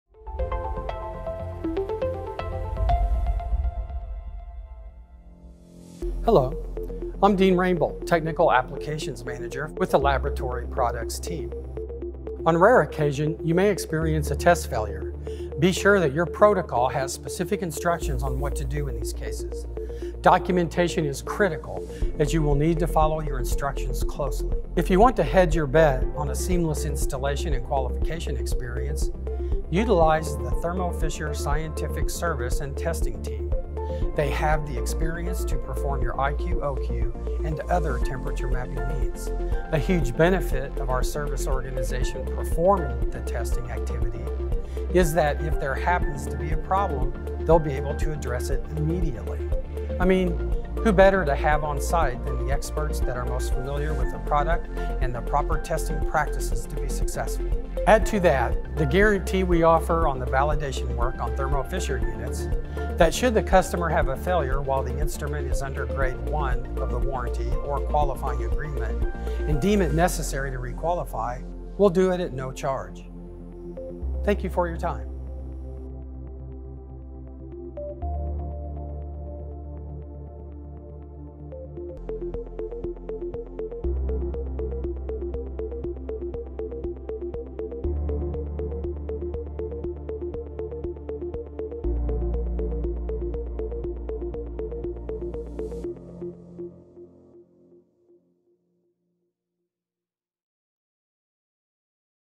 Regulatory & QC: Compliance services for Thermo Scientific™ lab equipment
This video explains the benefit of using compliance services  along with the purchase of equipment at POS, our  factory trained expert engineers will provide best in class service for customers operating in the regulated environment.

0:00 Introduction
0:36 Service and testing team
1:23 Validation guarantee
1:39 Conclusion